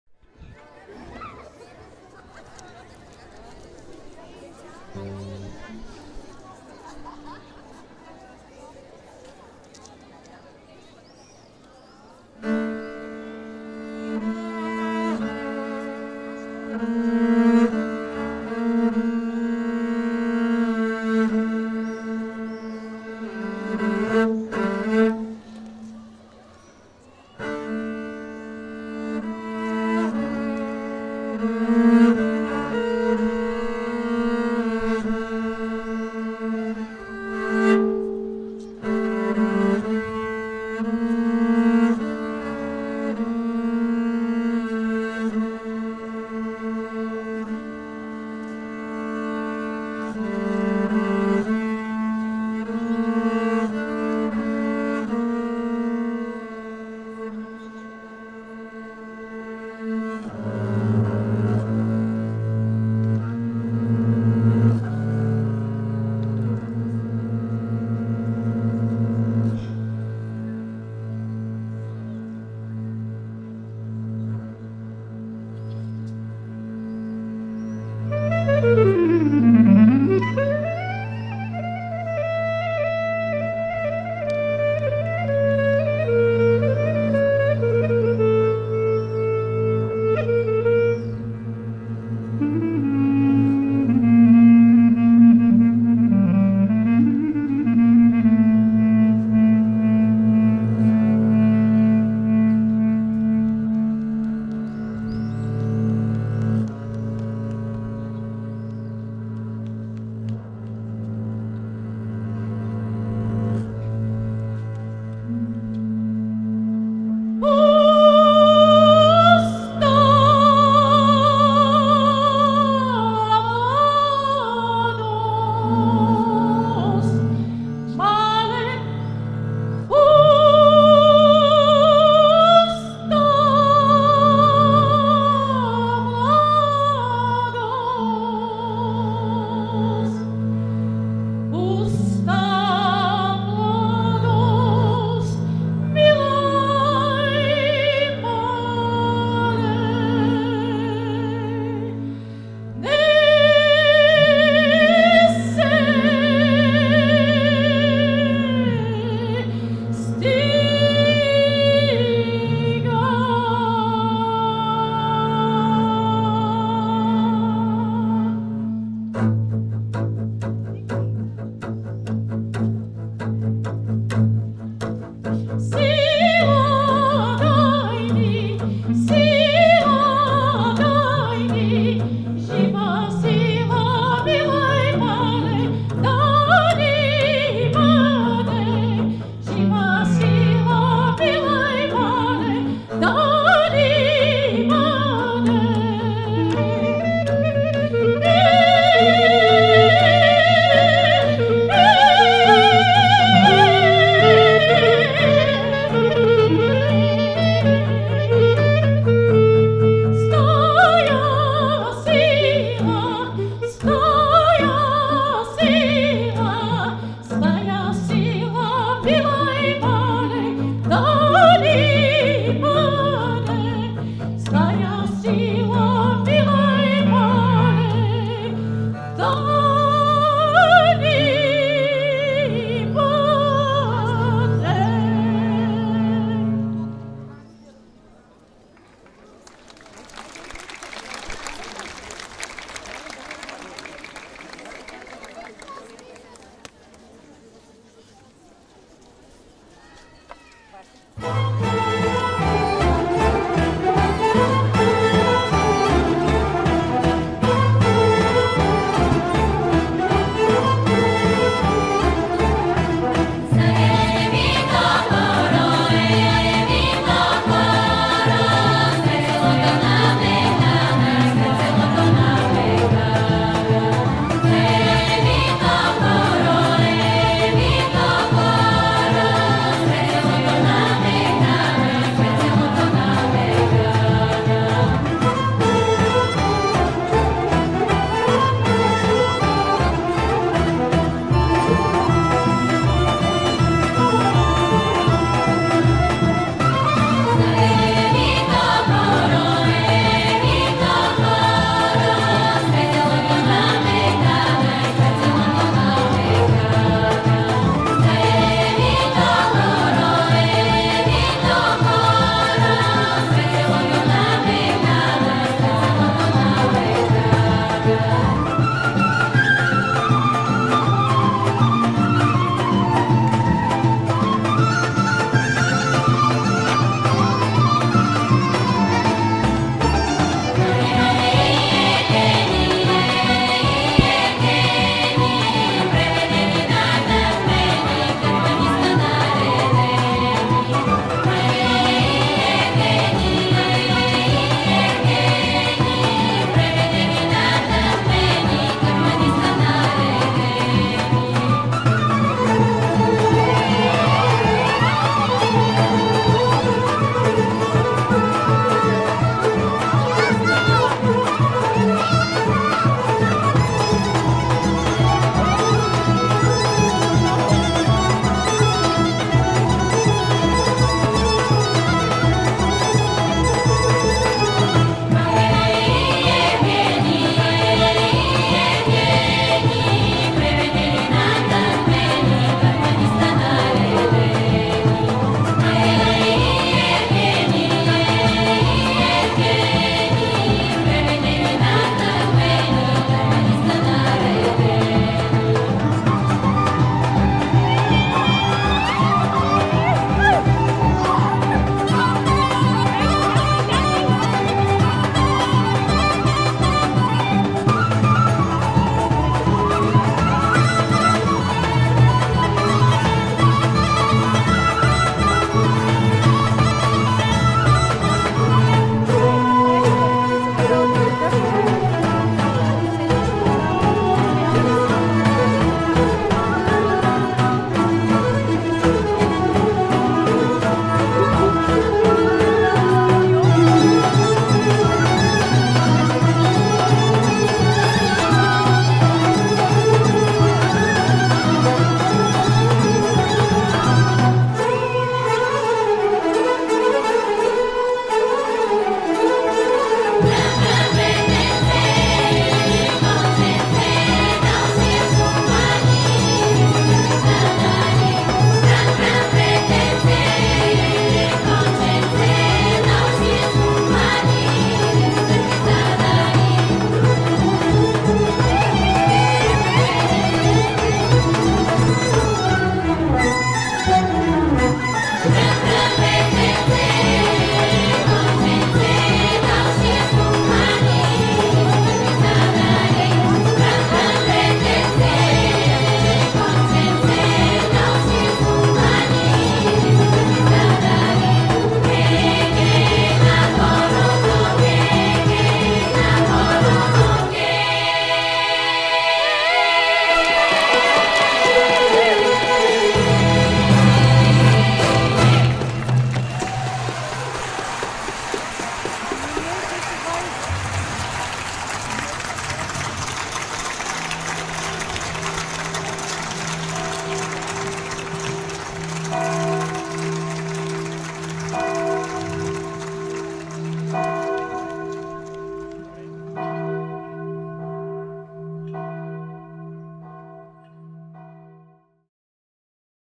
Flash Mob in Larnaca, Cyprus | "The Roses of Bulgaria" (11th May 2018)'' Розите на България'' Кипър
This flash mob was an event organized in Larnaca (Cyprus) to promote the "The Roses of Bulgaria" – an initiative by the Ministry of Education of the Republic of Bulgaria on the occasion of the day of holy brothers St. St. Cyril and Methodius.
The initiative aimed to attract the attention and popularize the Bulgarian folk dance and art abroad. The Larnaca flashmob was a memorable experience for the participants and spectators.
More than 100 Bulgarians and Cypriots danced together to the Bulgarian traditional music.

The children from the school "Dr Petar Beron" and we, their teachers, would like to thank to the people that helped us succeed:

Hristo Ovcharov - double bass
Svetlomir Dimitrov – clarinet
Tsveta Christophoru – vocal
The Bulgarian Schools in Cyprus: “Nikola Vaptsarov”, Nicosia, “Narodni Buditeli”, Paralimni, "Paisii Hilendarski", Limassol, "Rodna Striaha", Aradipu
“Iliachtida” Aesthetic Gymnastics Group
"Sovatchii" Dance Formation, Limassol
“Ethnika” Folk Dance Group, Paralimni
“Kanatitsa” Folk Dance Group, Nicosia and Larnaca.

Thank you Chris and Luisos! You were wonderful!